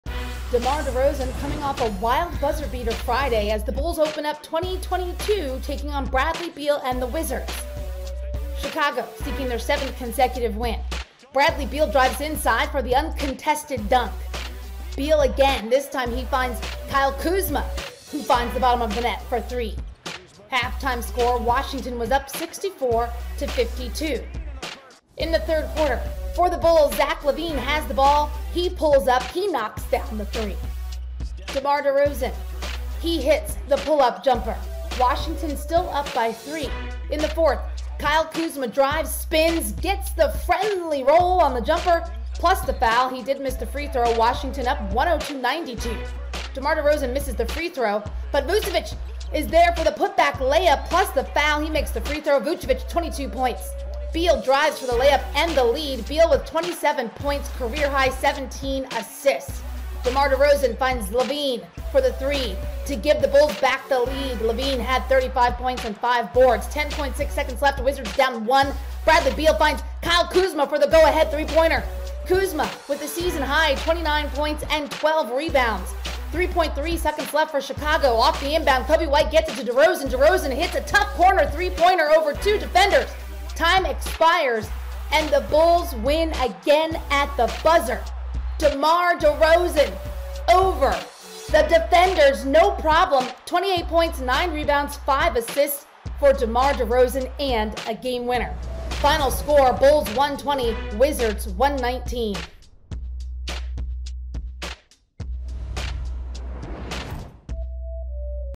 Game Recap: Bulls 120, Wizards 119
After Kyle Kuzma knocked down the go-ahead basket with 3.3 seconds remaining in regulation, DeMar DeRozan knocked down a corner three as time expired to lift the Bulls over the Wizards, 120-119, marking Chicago’s 7th consecutive victory. This is DeRozan’s 2nd consecutive game-winning basket at the final buzzer. Zach LaVine (35 points, 5 rebounds) and DeMar DeRozan (28 points, 9 rebounds, 5 assists) led the way for the Bulls, while Bradley Beal tallied 27 points and a career-high 17 assists for the Wizards in the losing effort. The Bulls improve to 24-10 on the season, while the Wizards fall to 18-18.